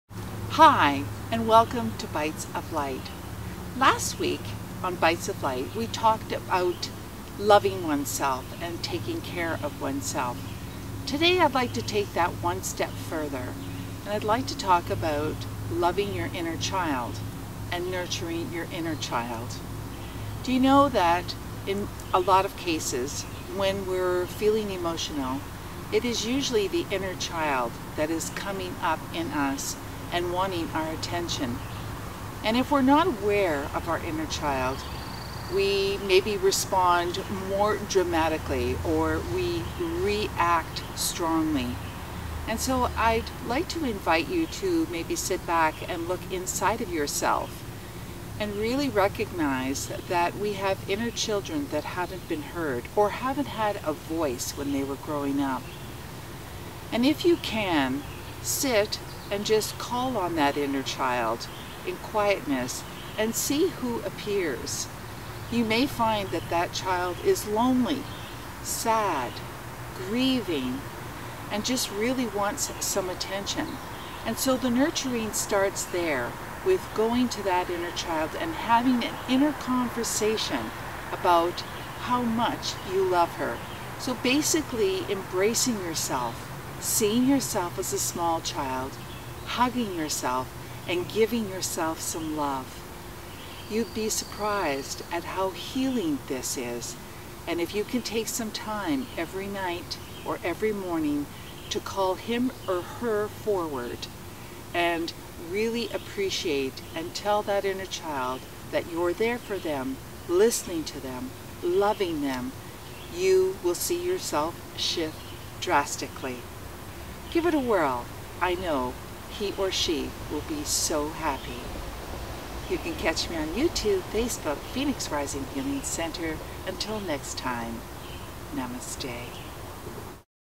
Bites of Light - Episode #39 Inner Child
A lot of times when we get emotional or reactive, it is our inner child trying to express itself. This is the time to sit in quietness and have a heart to heart with the little you.